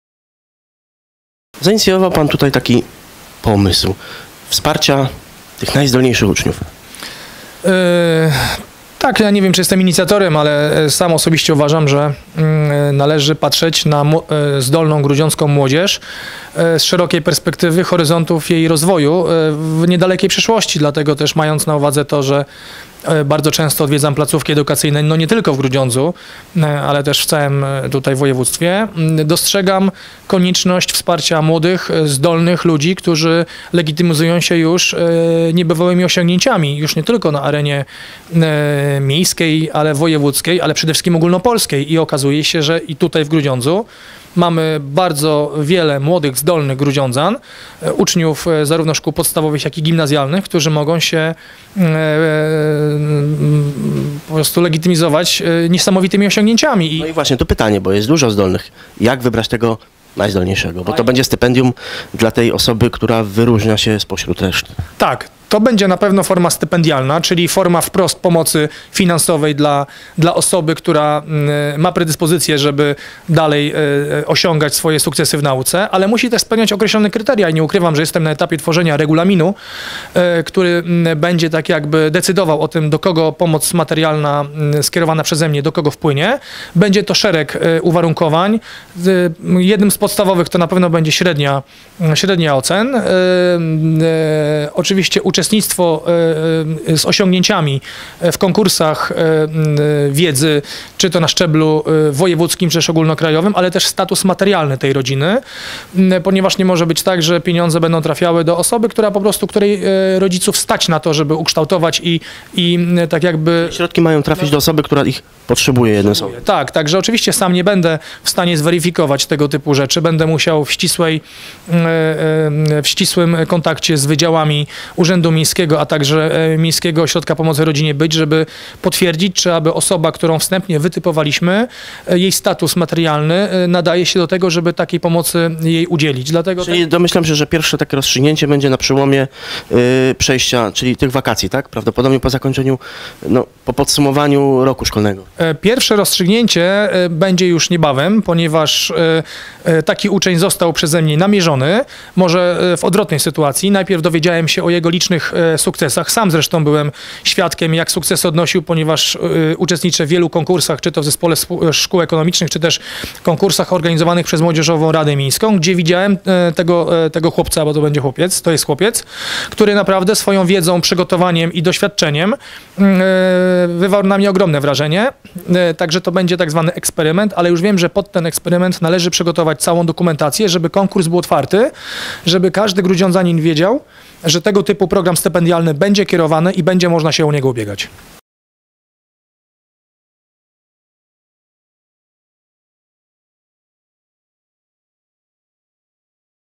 Stypendium dla najzdolniejszego ucznia
Zdaniem parlamentarzysty z Grudziądza ważnym kryterium tej pomocy będzie jej celowość, ponieważ pomoc materialna ma być skierowana do osób zdolnych, ale też takich, które jej potrzebują.